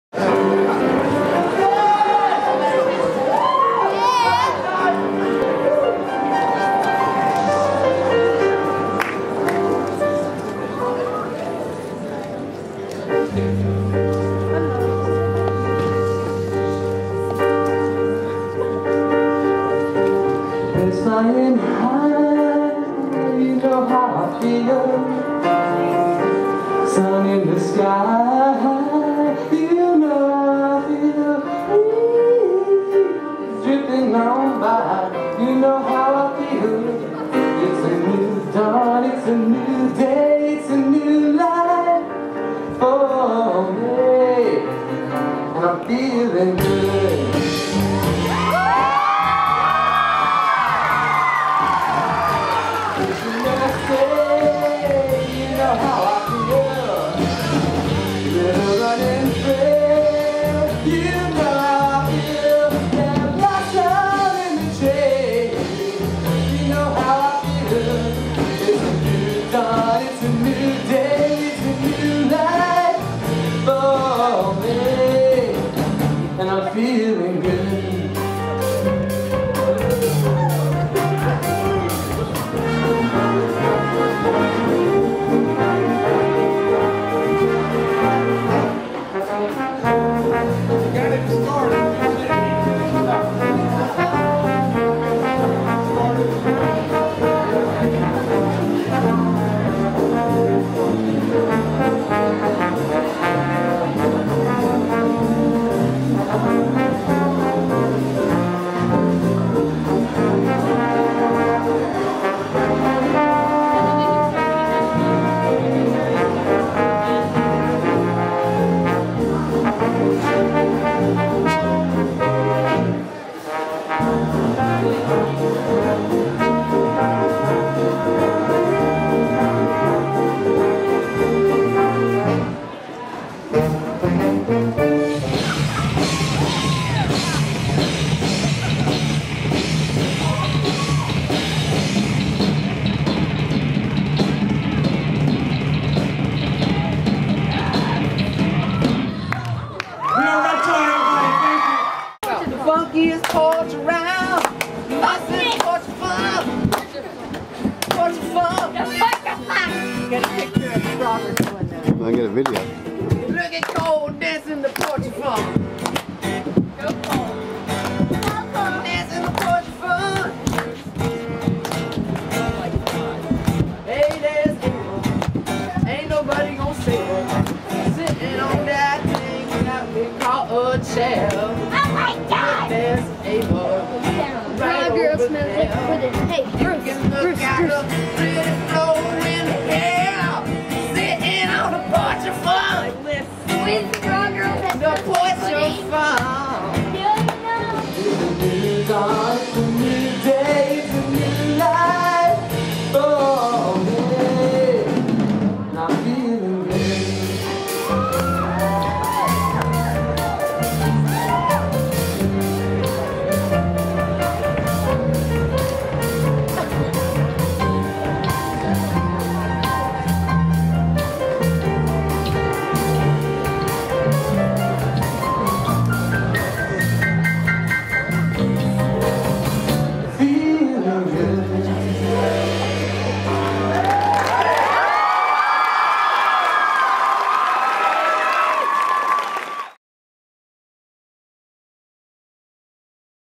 Robert Torrence - Music Clips
Robert Torrence
-- piano and vocals (PHHS Battle of the Bands, 2010)
-- trombone (PHHS Jazz Ensemble, 2010)
-- drums (PHHS Battle of the Bands, 2010)
-- guitar, harmonica, and vocals (porch of funk, 2010)